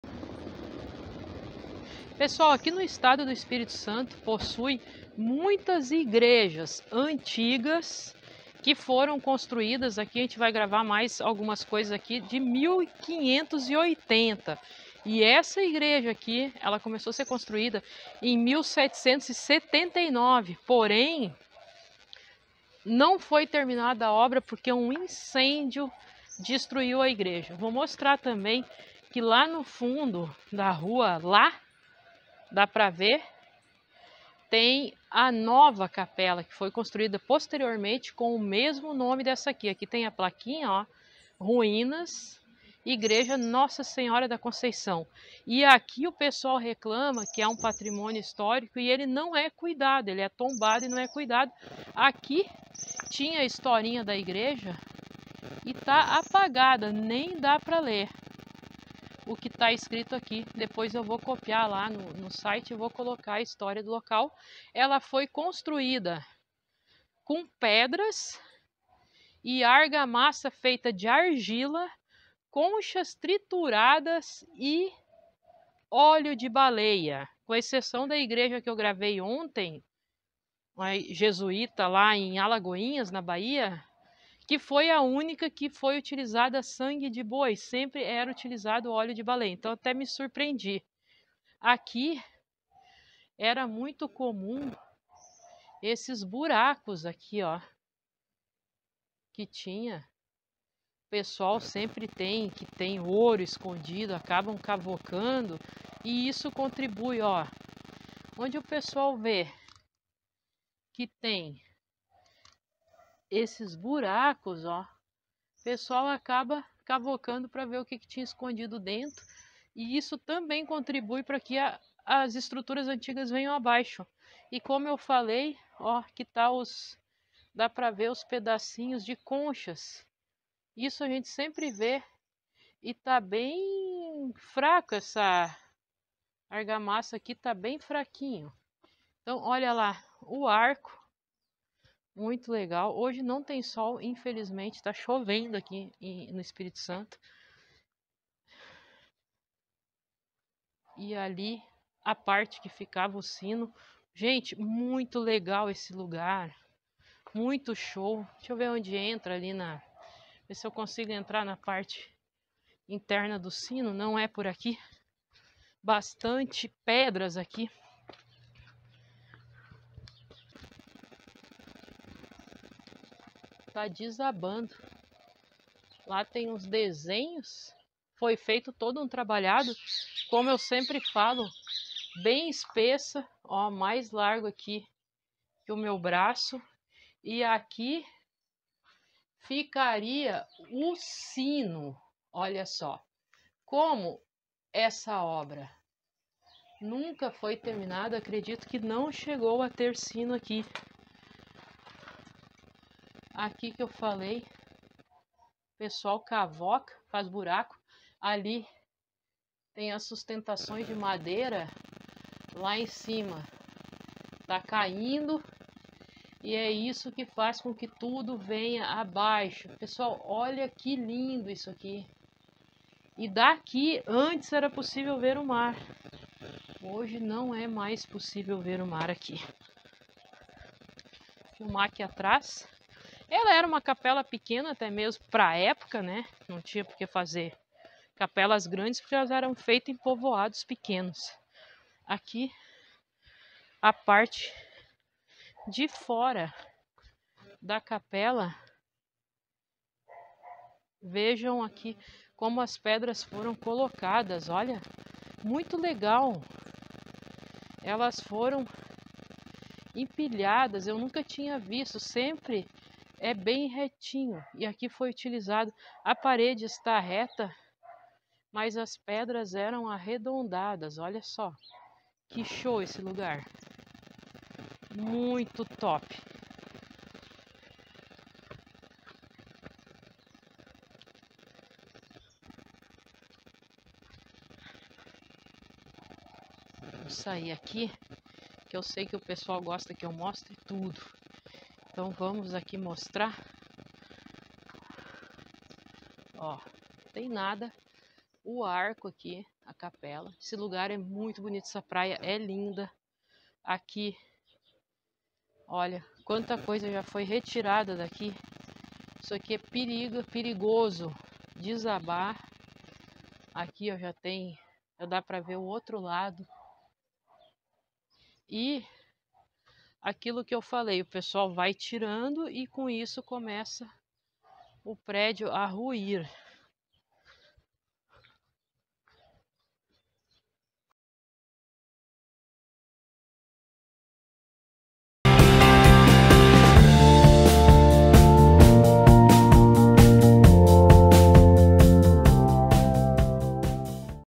IGREJA ABANDONADA EM RUINA DE 1779
Ruínas de uma igreja cuja construção iniciou-se em 1779 e que apesar de tombada está em total abandono. Fica na cidade de Guarapari, no Espírito Santo.